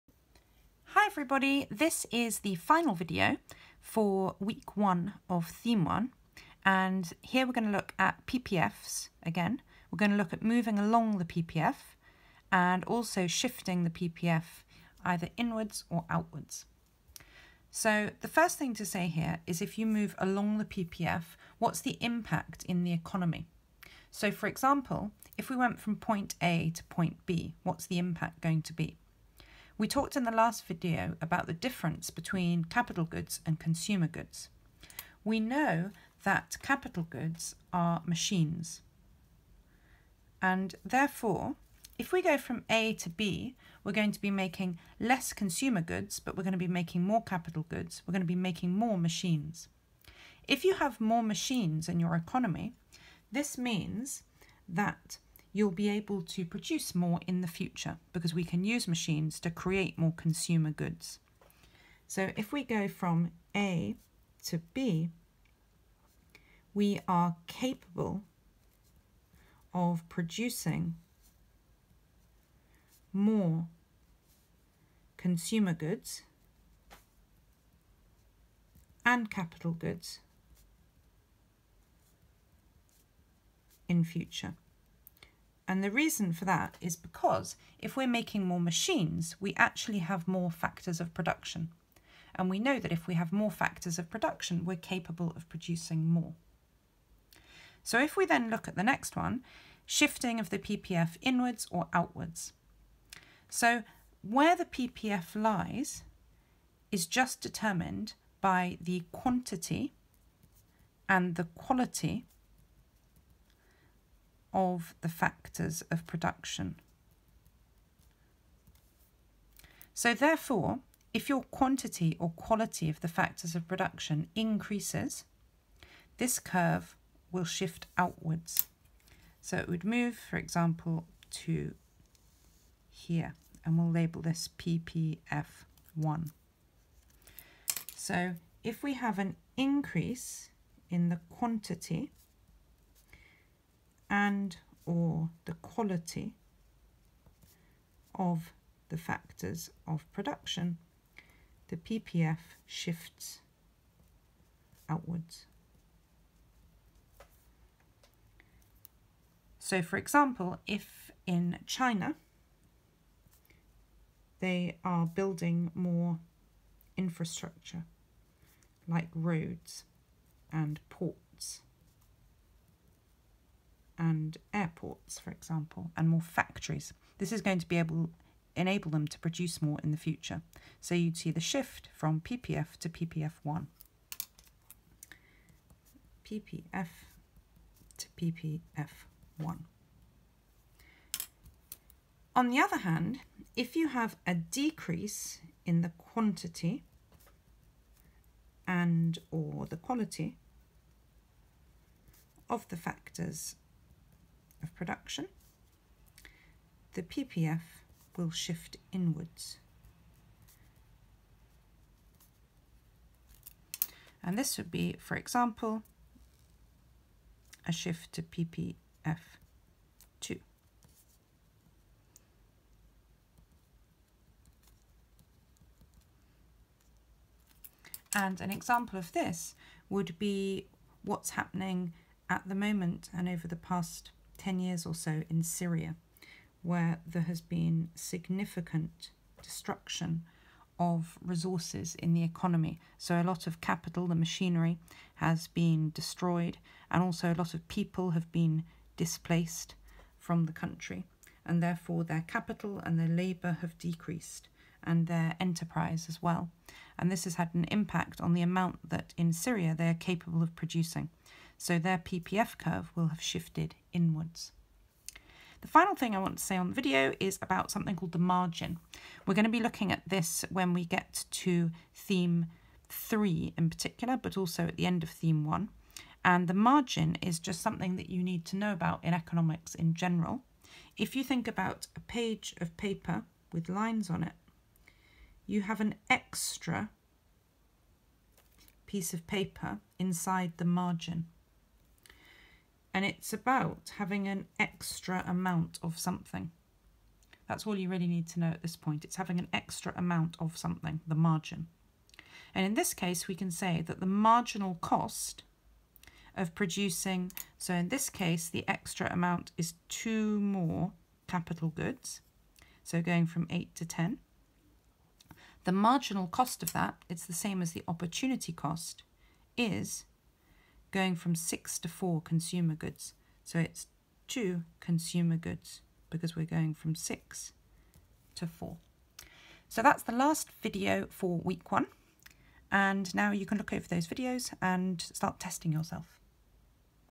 PPFs - moving along and shifting the PPF
Explaining in simple terms what it means to move along a production possibility frontier and how this is different to shifting the whole curve